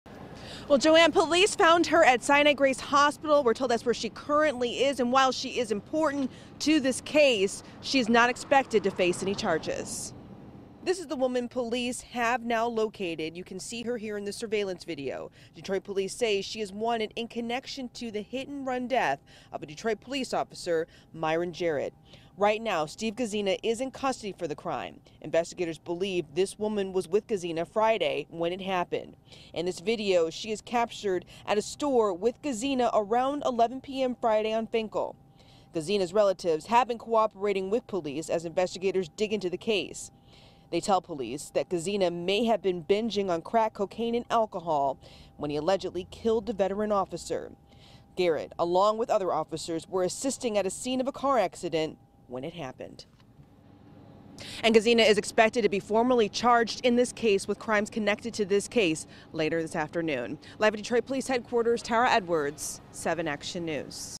Detroit Police looking for woman who is a witness to a deadly accident that killed officer
Detroit Police looking for hit and run witness in officer's death